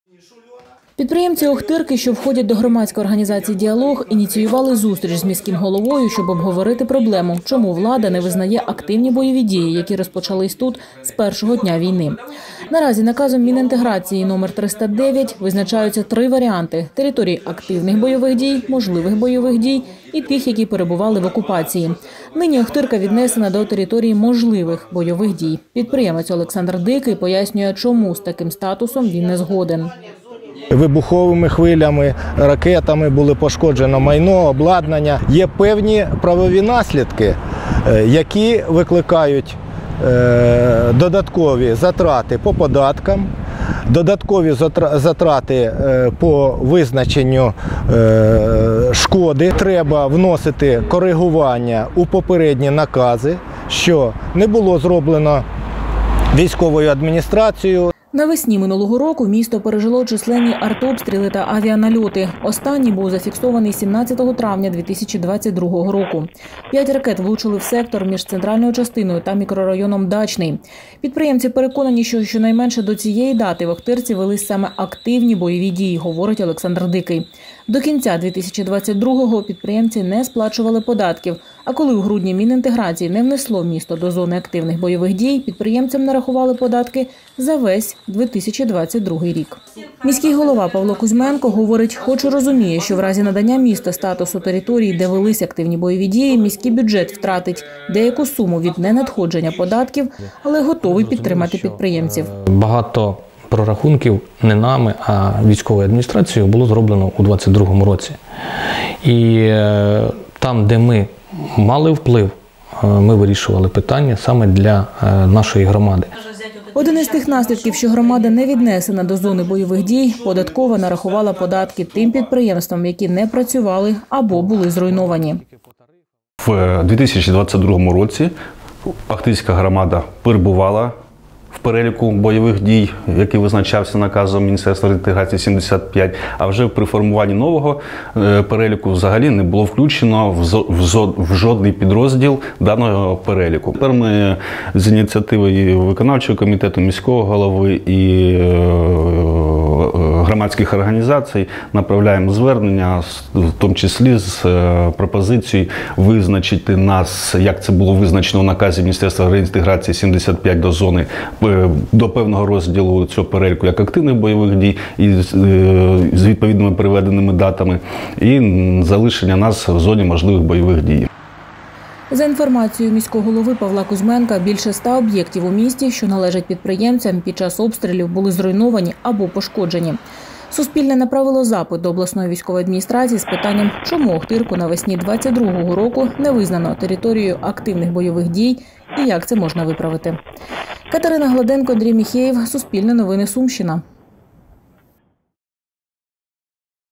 Підприємці Охтирки борються за те, щоб місто віднесли до територій, де велись активні бойові дії
Підприємці Охтирки, що входять до ГО “Діалог”, ініціювали зустріч з міським головою, щоб обговорити проблему, чому влада не визнає активні бойові дії, які розпочалися тут з першого дня війни. Наразі, наказом Мінреінтеграції № 309 від 22 грудня 2022 року визначаються три варіанти: території активних бойових дій, можливих бойових дій і тих, які перебували в окупації. Нині Охтирка віднесена до території можливих бойових дій.